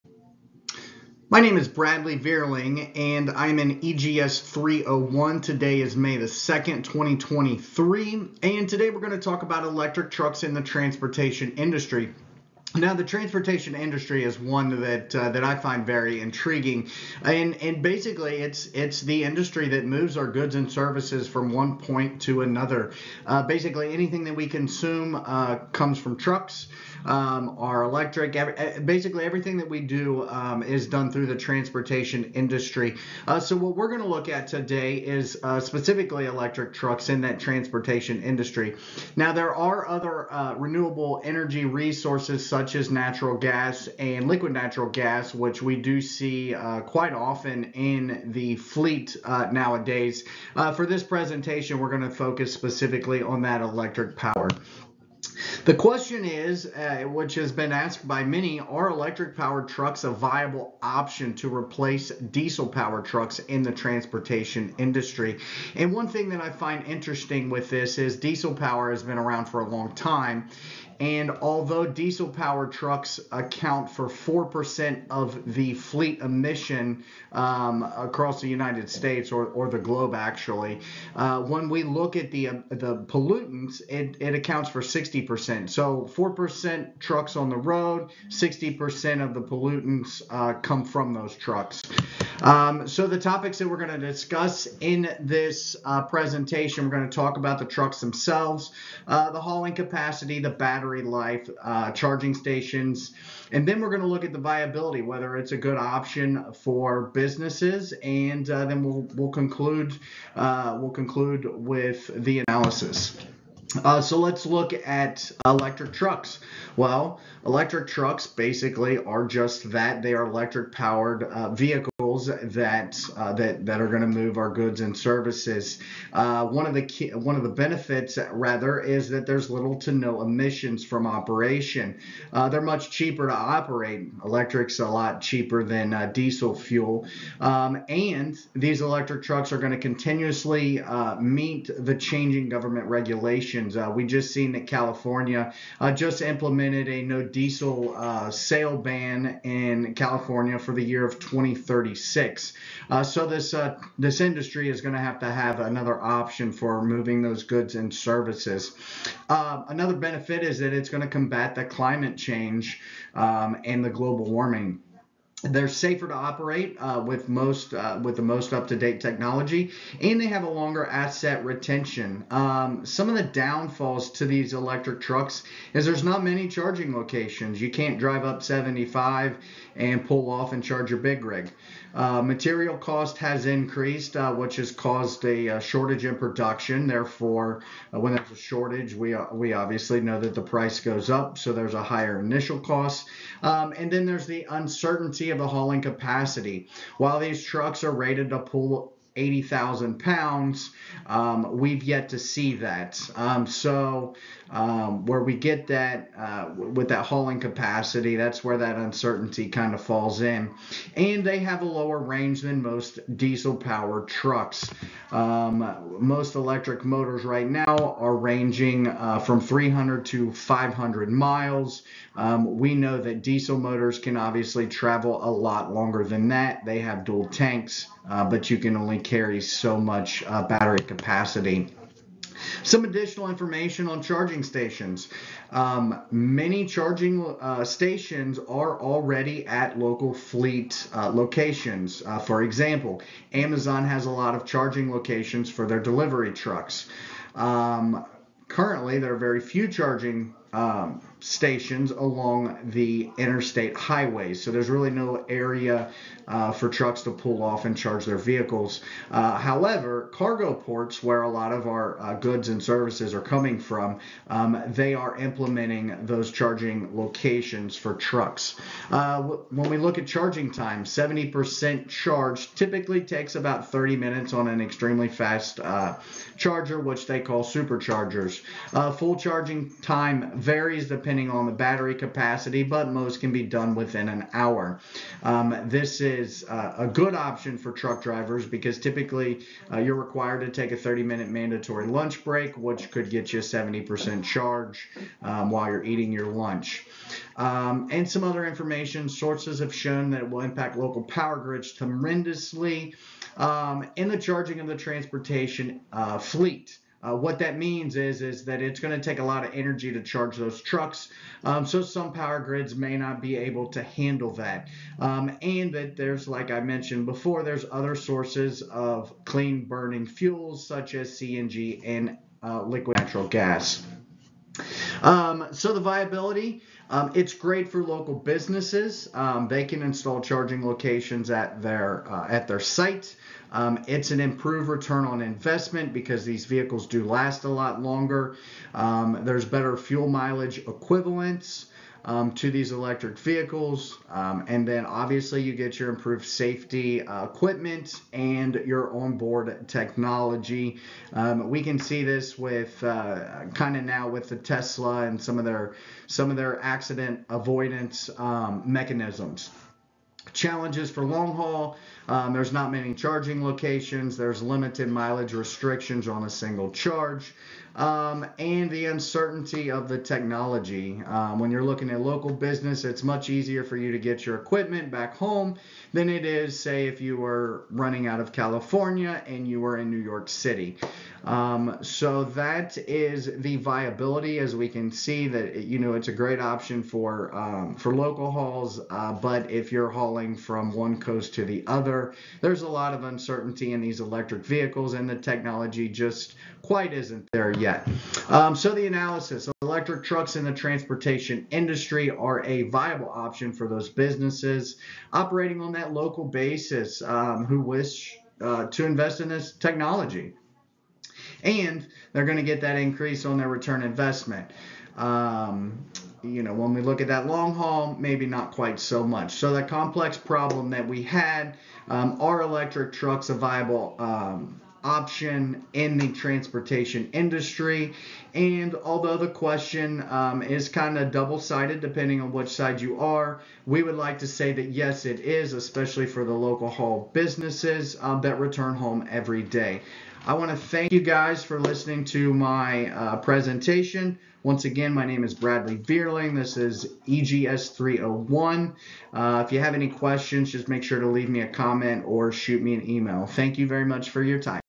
Electric Trucks in the Transportation Industry
EGS 301 Student Presentation - Spring 2023

Bibliography
Ahmed, Hassan Yousif, et al. “A Multi-Objective Planning Strategy for Electric Vehicle Charging Stations towards Low Carbon-Oriented Modern Power Systems.” Sustainability (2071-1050), vol. 15, no. 3, Feb. 2023, p. 2819. EBSCOhost,

Angel Alejandro Juan, et al. “Electric Vehicles in Logistics and Transportation: A Survey on Emerging Environmental, Strategic, and Operational Challenges.” Energies, vol. 9, no. 2, Jan. 2016, p. 86. EBSCOhost,

Letnik, Tomislav, et al. “Impact of Logistics Trends on Freight Transport Development in Urban Areas.” Sustainability (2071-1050), vol. 14, no. 24, Dec. 2022, p. 16551. EBSCOhost,

Lopez, Frank. “Trucking Industry Eyes an Electric Future in Face of Regulation.” Business Journal Serving Fresno & the Central San Joaquin Valley, no. 3259310, Sept. 2022, p. 10. EBSCOhost,

Peng, Dongbo, et al. “Bi-Level Fleet Dispatching Strategy for Battery-Electric Trucks: A Real-World Case Study.” Sustainability (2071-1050), vol. 15, no. 2, Jan. 2023, p. 925. EBSCOhost,

Teimoori, Zeinab, and Abdulsalam Yassine. “A Review on Intelligent Energy Management Systems for Future Electric Vehicle Transportation.” Sustainability (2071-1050), vol. 14, no. 21, Nov. 2022, p. 14100. EBSCOhost,

Zähringer, Maximilian, et al. “Time vs. Capacity—The Potential of Optimal Charging Stop Strategies for Battery Electric Trucks.” Energies (19961073), vol. 15, no. 19, Oct. 2022, p. 7137. EBSCOhost,